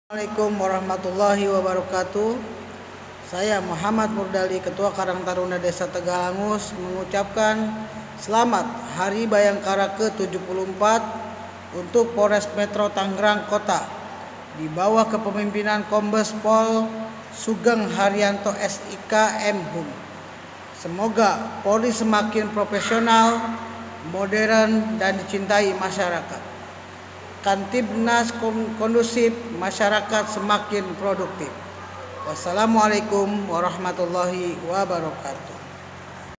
Ucapan hut bayangkara ke 74 oleh murdali (ketua karang taruna desa tegalangus)
Video dari Abi Shipa & Raikhan